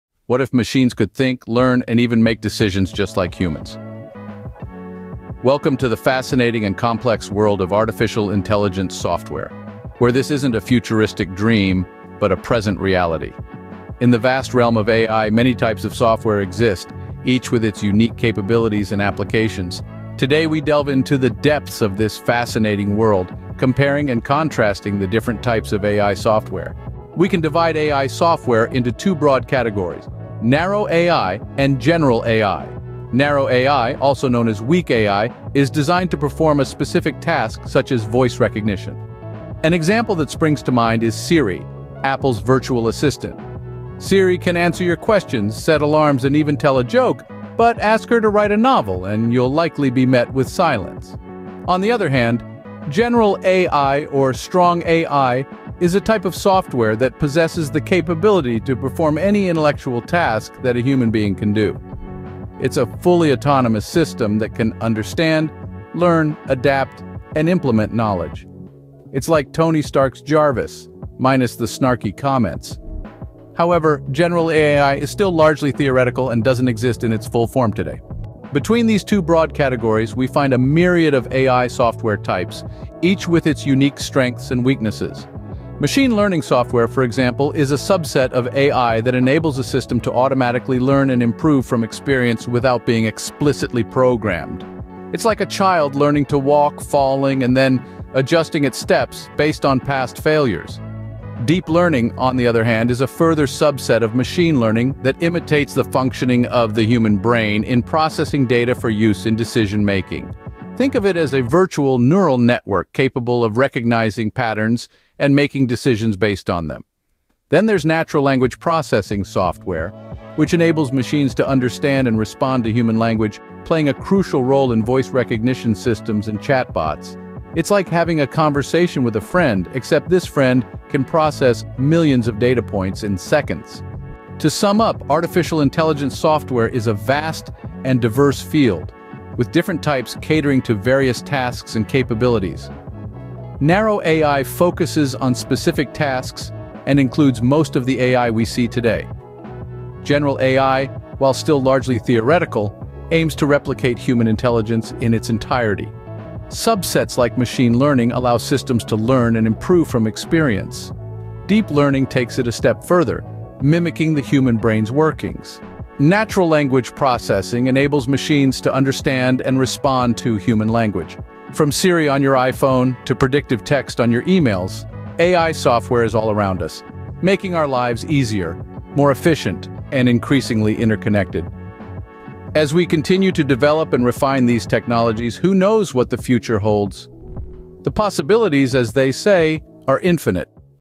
Unraveling the Types of AI Software
Unraveling the Types of AI Software by LaSoft

OUTLINE:

00:00:00 Introduction to Artificial Intelligence
00:00:26 Narrow AI vs. General AI
00:01:18 Types of AI Software
00:02:14 Conclusion and Future of AI